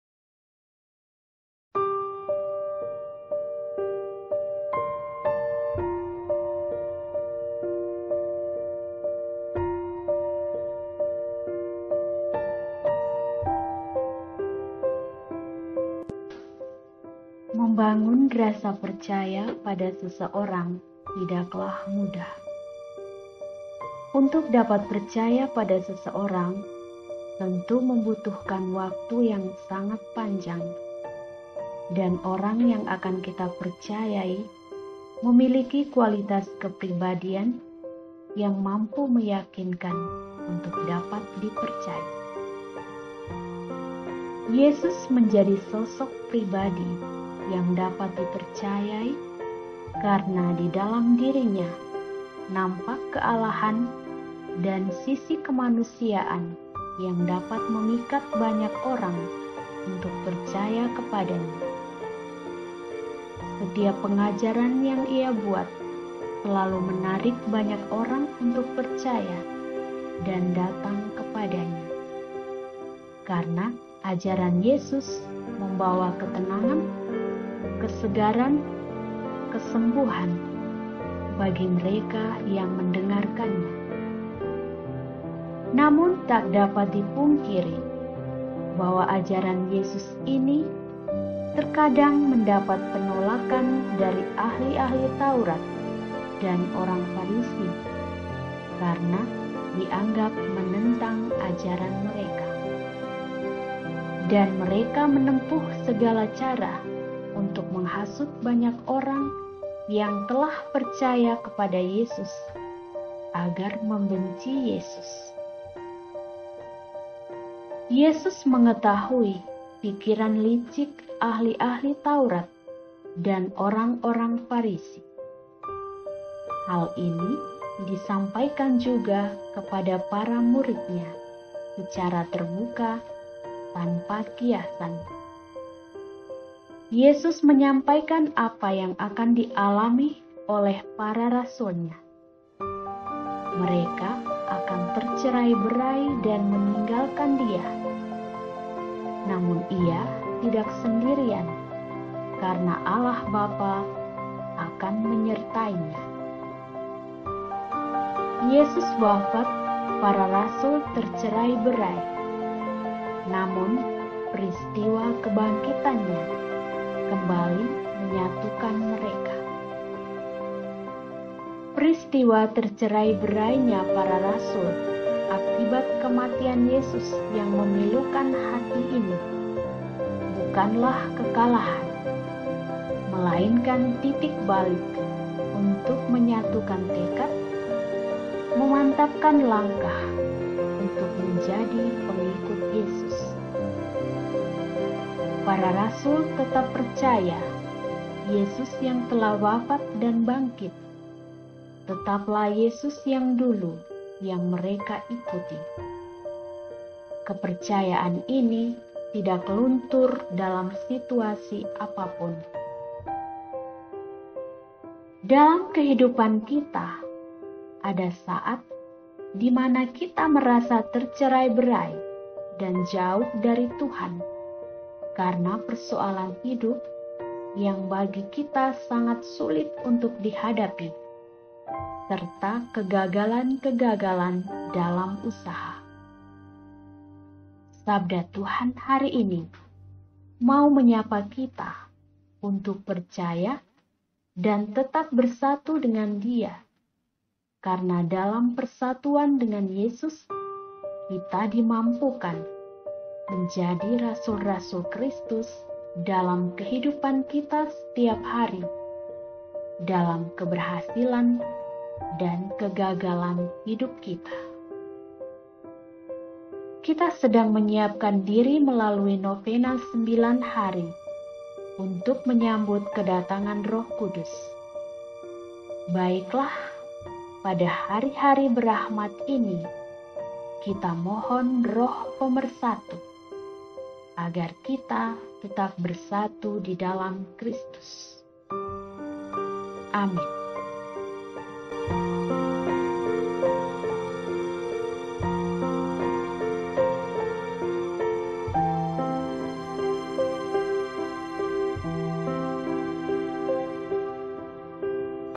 Kuatkanlah Hatimu- ( Yohanes, 16 : 29 -33) Renungan harian , Senin, 17 Mei 2021
Renungan harian Katolik ( @Provinsi SSpS Flores Bagian Timur)